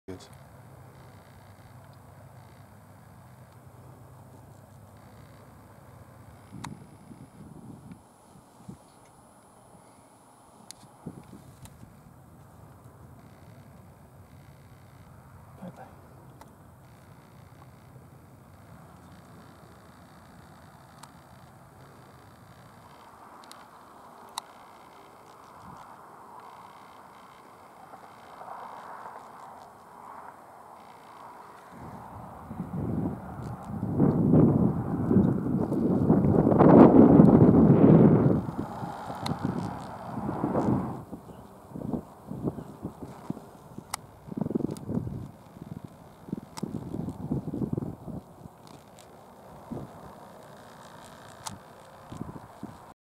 Roadrunner catching Grasshoppers
Short video of our Roadrunner jumping, running and catching grasshoppers.